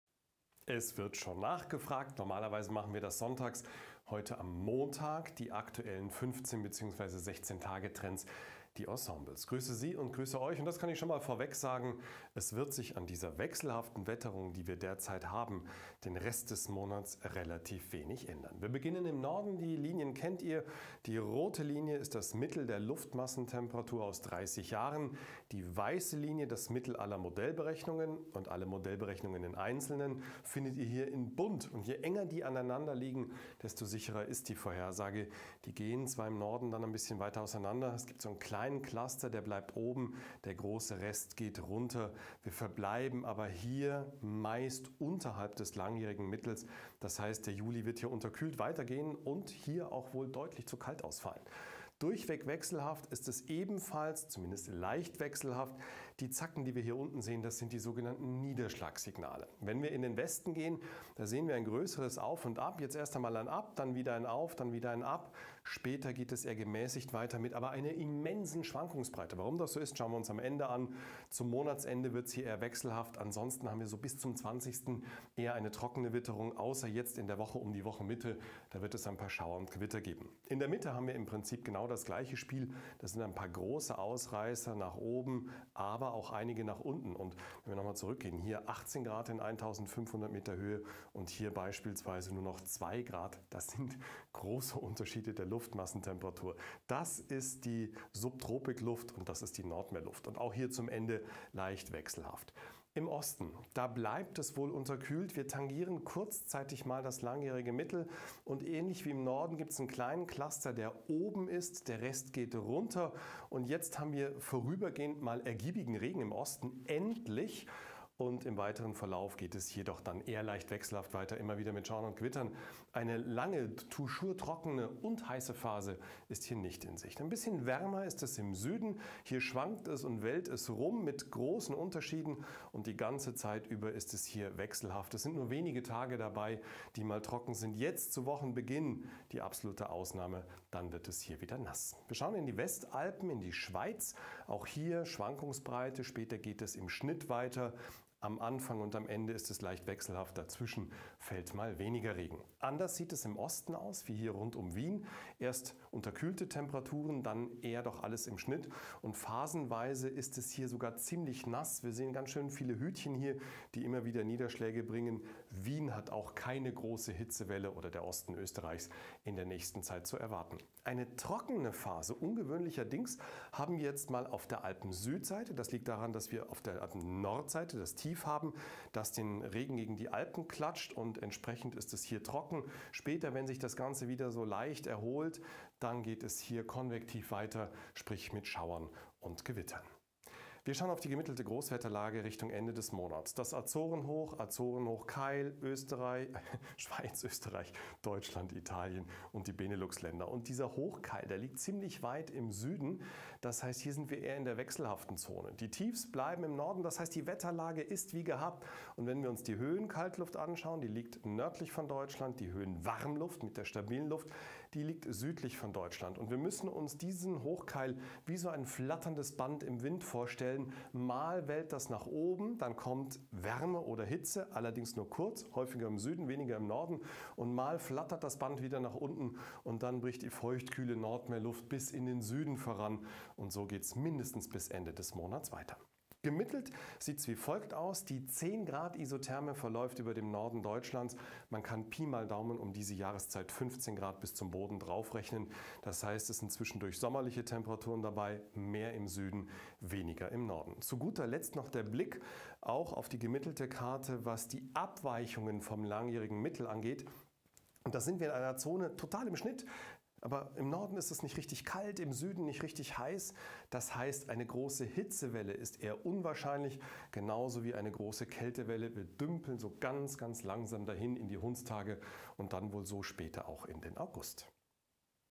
Aktuelle 16 Tage-Trends: Wie geht der Juli weiter? Bringt der Beginn der Hundstage Hochsommerwetter?
Die aktuellen Ensembles, also die 15 oder 16 Tage-Trends sprechen eine eindeutige Sprache: Es bleibt mehr oder weniger alles so wie gehabt: Der Norden verbleibt mehr in kühlerer Luft, der Süden bekommt phasenweise Mittelmeerluft und alles ist und bleibt im Wechsel. Größere und vor allem beständige Ausreißer in eine bestimmte Richtung sind nicht in Sicht. Zwischendurch wird es auch immer wieder Regen geben – keine Wahnsinns-Summen, dafür aber auch keine dauerhafte Trockenheit.

Immerhin kommen wir schon in den Bereich der Hundstage – klassischerweise die heißesten Tage des Jahres. Der Start dieser Zeit (23. Juli bis 23. August) verläuft allerdings nicht allzu heiß…